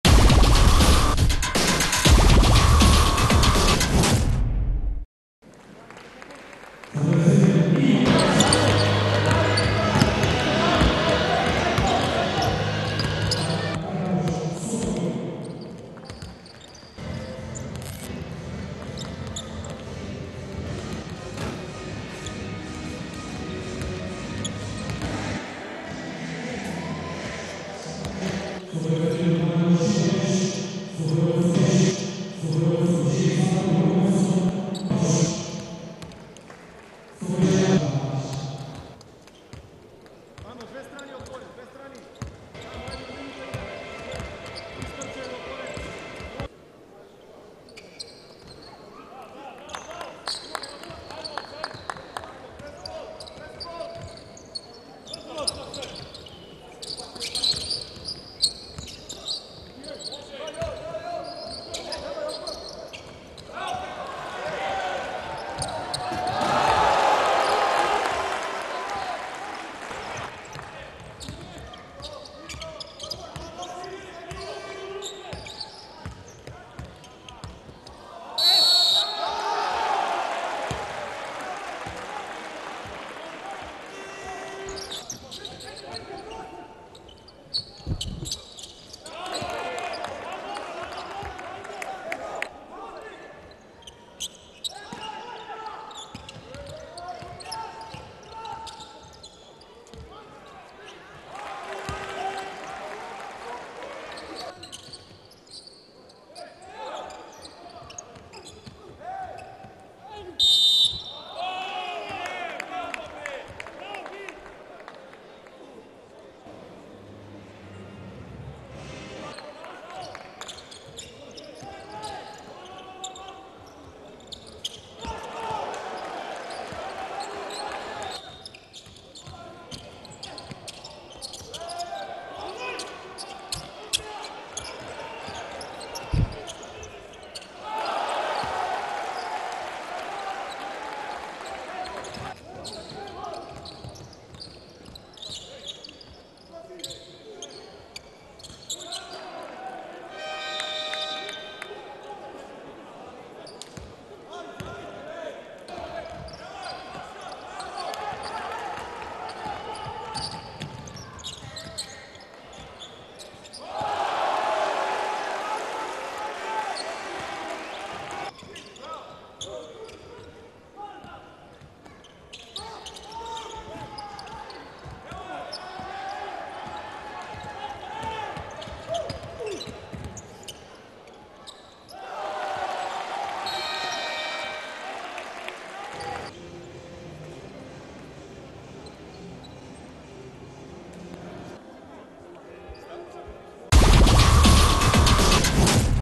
ABA Liga 2016/17 highlights, Round 2: Karpoš Sokoli - Igokea (5.10.2016)
Take a look at the highlights of 2016/17 season ABA League game between Karpoš Sokoli and Igokea in S.C. Boris Trajkovski, Skopje.